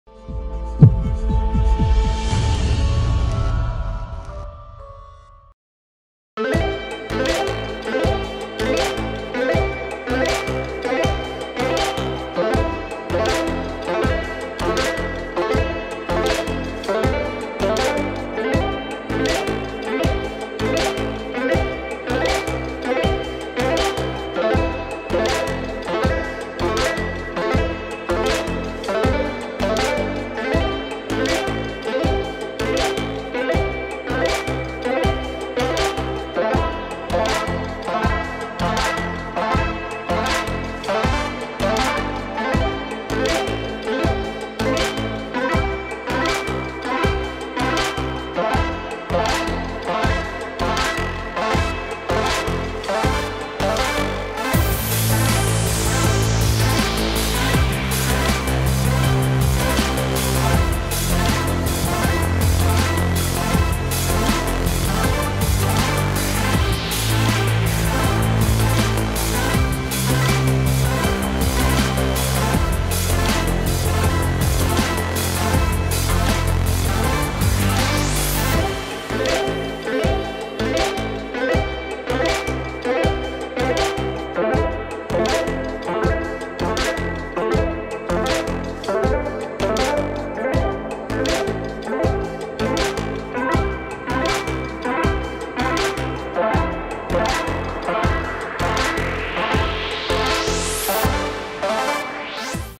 LNF2019 - Gols - Oitavas Ida - Foz Cataratas 3x2 Campo Mourão
Gols:
Foz Cataratas - Henrique e Trentin (2)
Campo Mourão - Caio e Luiz